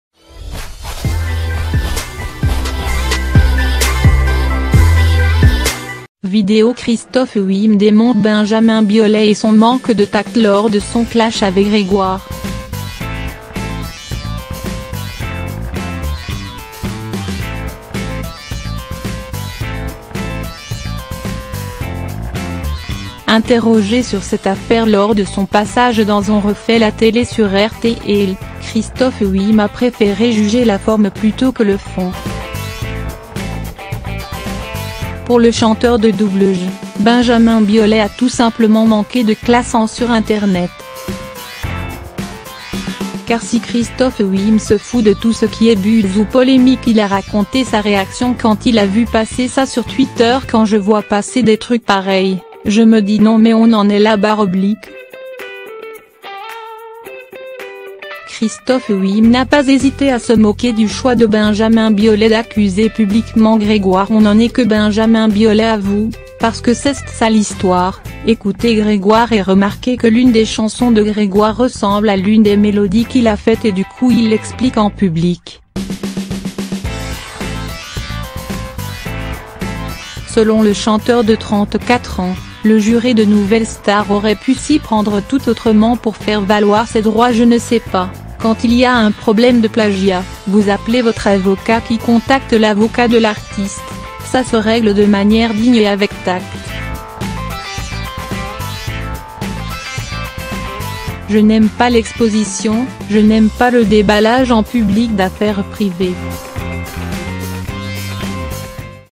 VIDEO Chris­tophe Willem démonte Benja­min Biolay et son manque de tact lors de son
Invité sur RTL, Chris­tophe Willem n'a pas été tendre avec Benja­min Biolay, qui avait récem­ment accusé publique­ment le chan­teur Grégoire de plagiat.